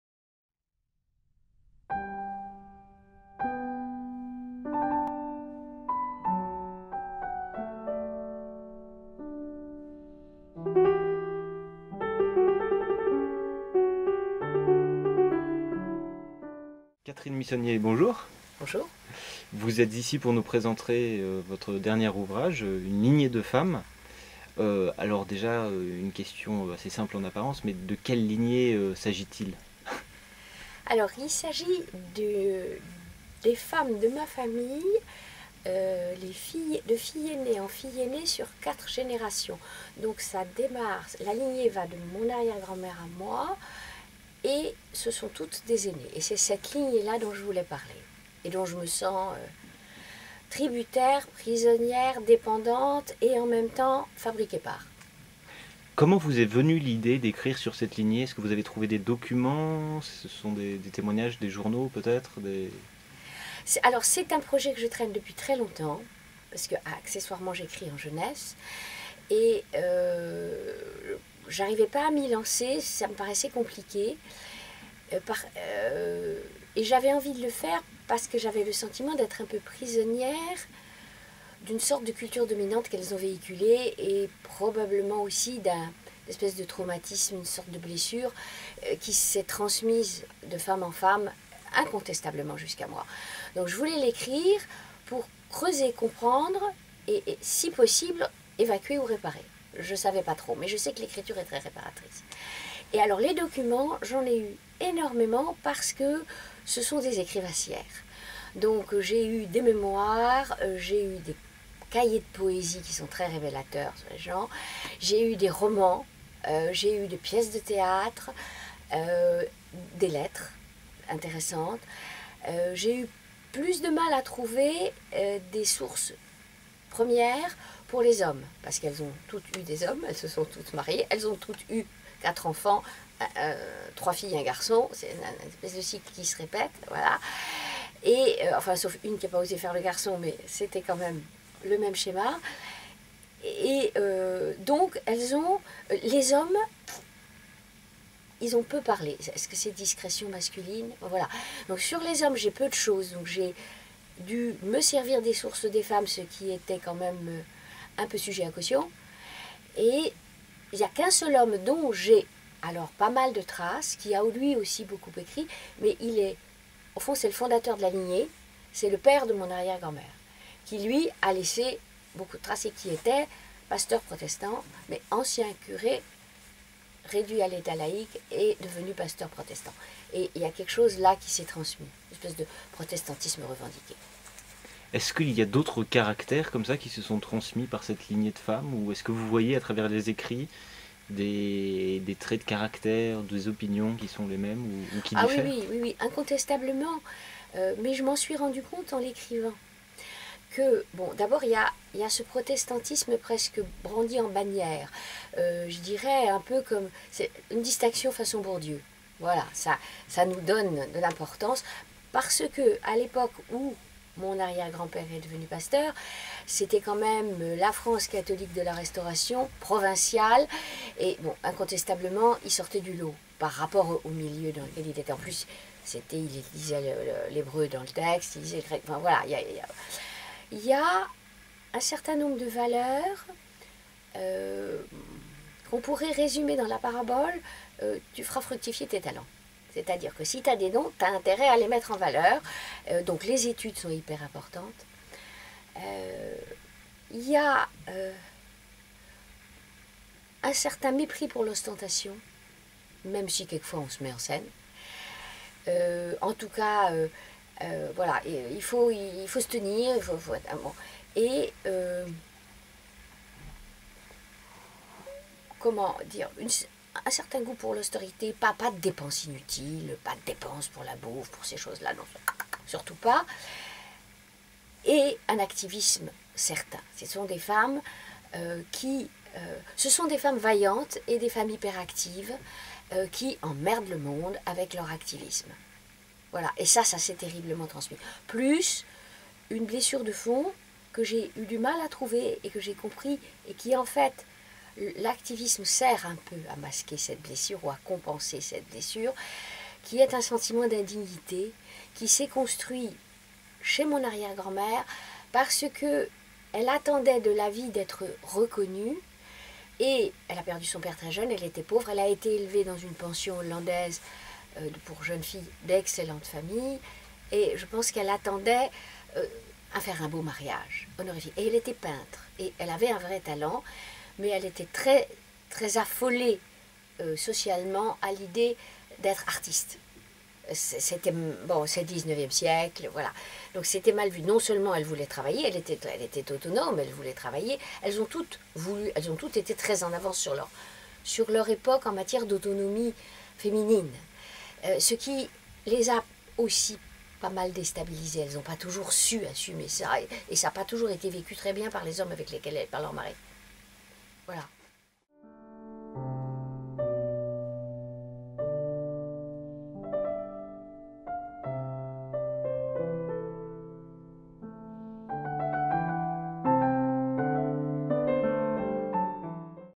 UNE LIGNÉE DE FEMMES - Catherine Missonnier
UNE LIGNÉE DE FEMMES

Peut-on échapper à l'emprise de sa mère ?

Catherine Missonnier

TÉMOIGNAGE, AUTOBIOGRAPHIE, RÉCIT

Jusqu'à quel point sommes-nous le produit de notre histoire familiale. Avons-nous même une marge de liberté ? A travers le destin de quatre générations de femmes tentant à toute force de concilier leurs aspirations créatrices, leur soif de respectabilité et leur désir d'enfants, l'auteure décrypte la genèse du moule qui l'a façonnée.

Broché

ISBN : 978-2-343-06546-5 • novembre 2015 • 356 pages

Prix éditeur : 26 € 24,70 €